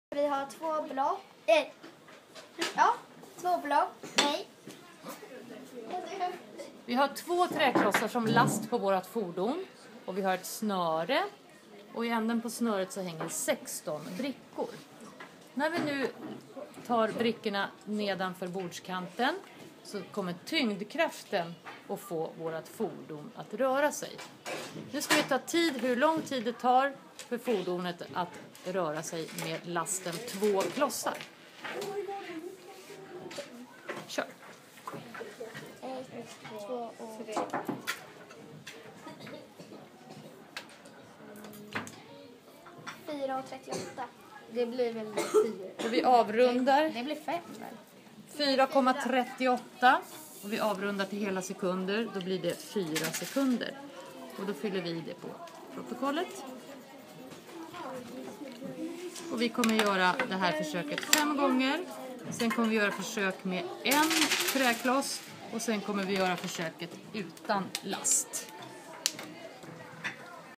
Rörelse och konstruktion uppdrag 3
Hur påverkar last fordonets hastighet?
Hur kan man koppla newtons 1:a lag  - Tröghetslagen till detta?